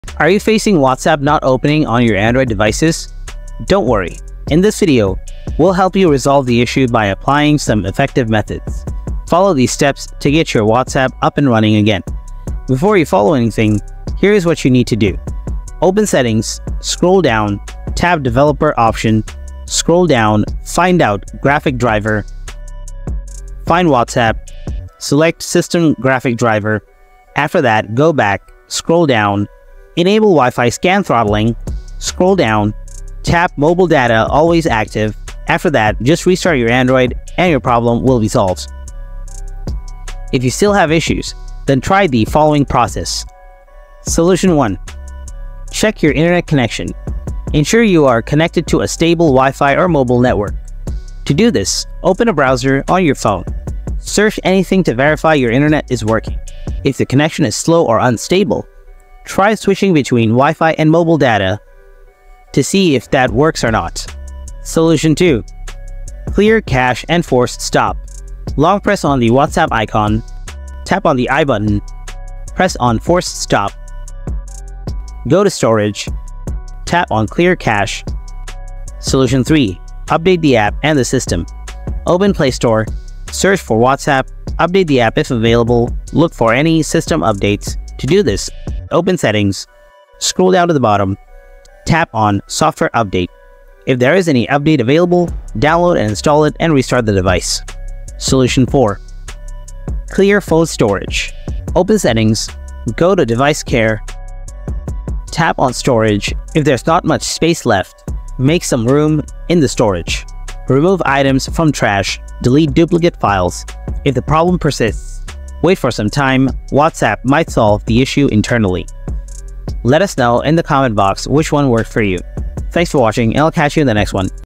How To Fix WhatsApp Not Opening Problem on Android | WhatsApp Keeps Crashing Solution on Android
Wondering why WhatsApp keeps stopping or not opening on your Android? If your Android WhatsApp closes right after opening it could be related to app cache, data corruption, outdated app versions, and so on.  But with the right method, you can easily troubleshoot the WhatsApp crashing issue!

So if you are still unable to open your WhatsApp Android, follow my steps in the video and by the time you are done, you will have solved the WhatsApp 'Not Open' error on your Android phone forever!

00:01- Video Intro
00:12- Basic Solutions
00:43- Solution 1: Check Internet
01:04- Solution 2: Clear Cache & Force Stop
01:20- Solution 3: Update the App & System
01:38- Solution 4: Clear Phone Storage
01:58- Solution End